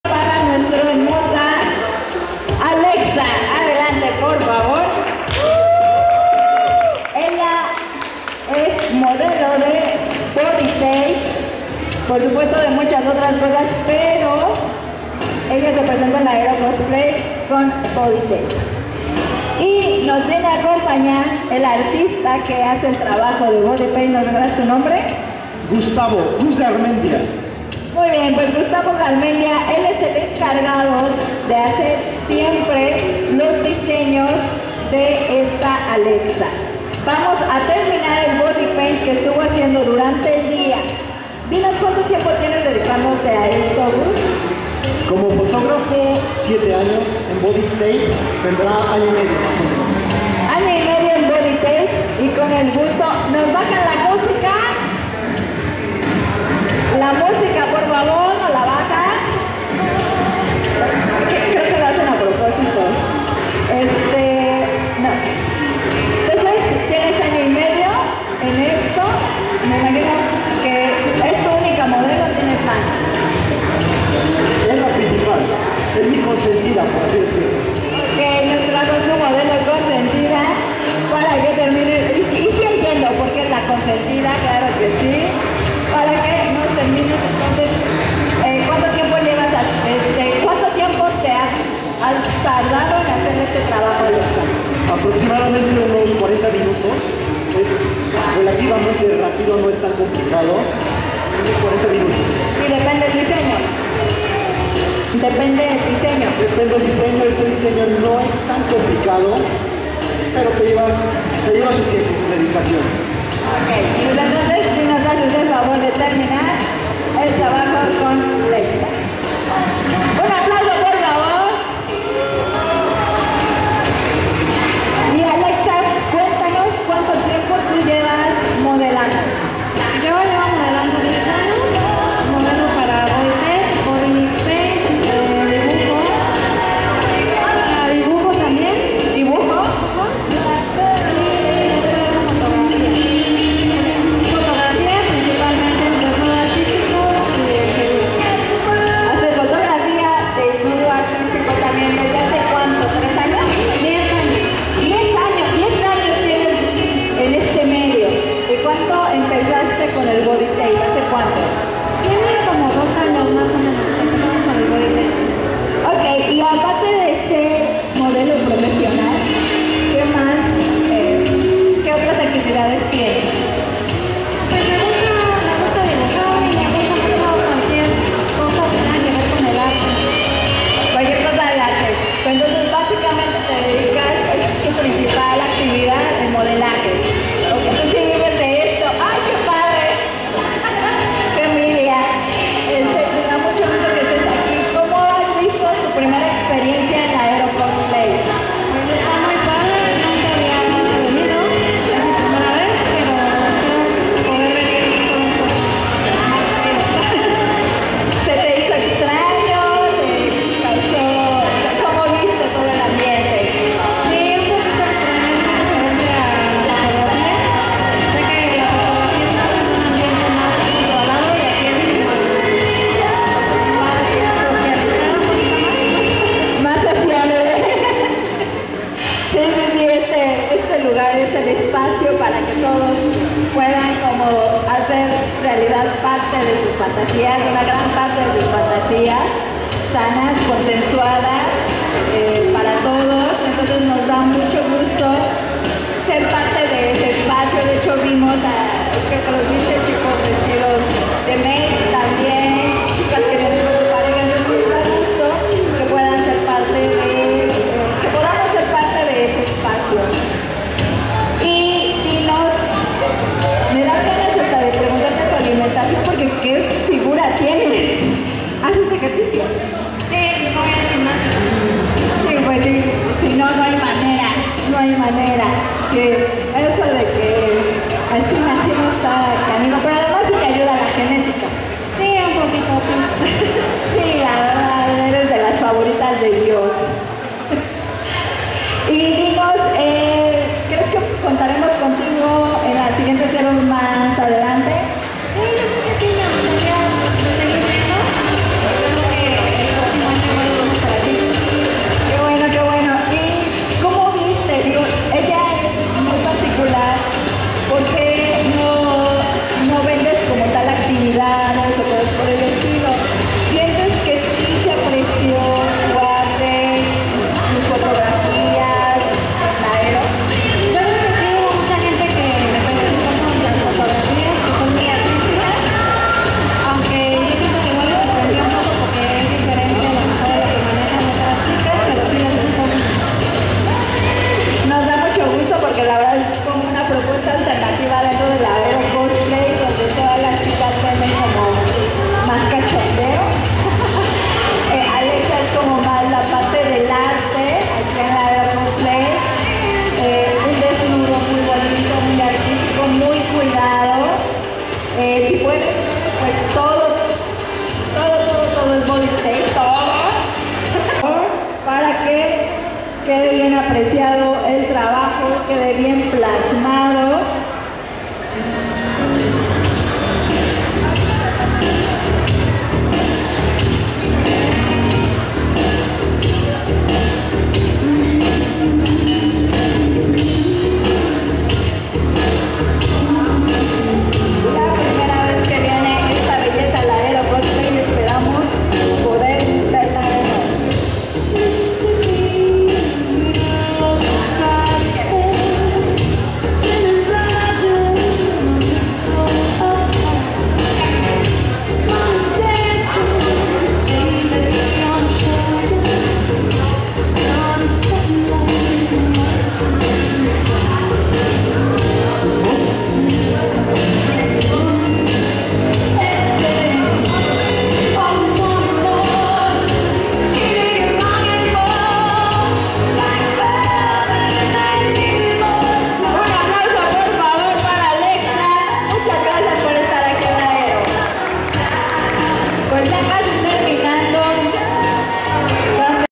Alexxa en la Ero Cosplay Festival CDMX edición "Maids"
Alexxa en la Ero Cosplay Festival CDMX edición "Maids", esto el dia 17 de agosto 2024.